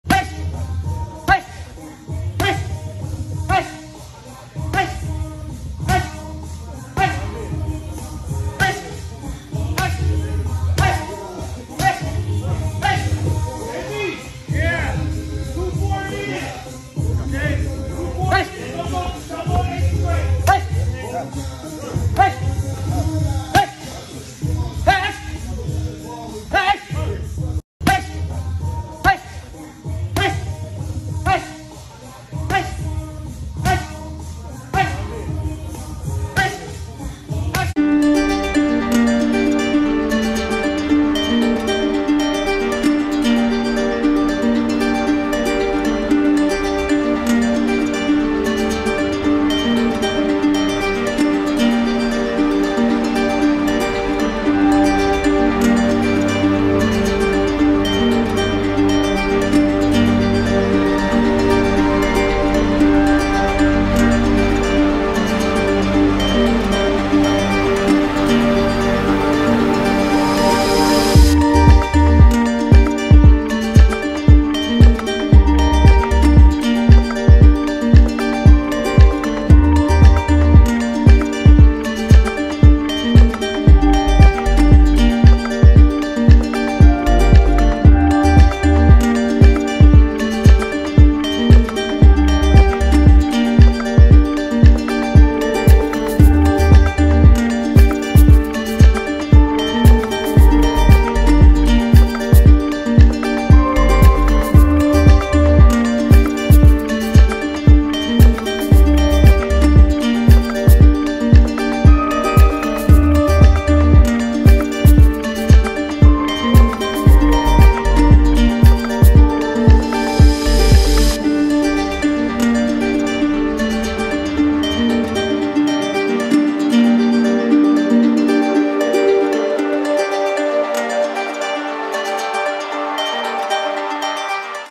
Terence Crawford Training Amir Khan on the Mitts for Kell Brook Fight
Boxing 🥊 highlights 🗞: Terence Crawford Training Amir Khan on the Mitts for Kell Brook Fight.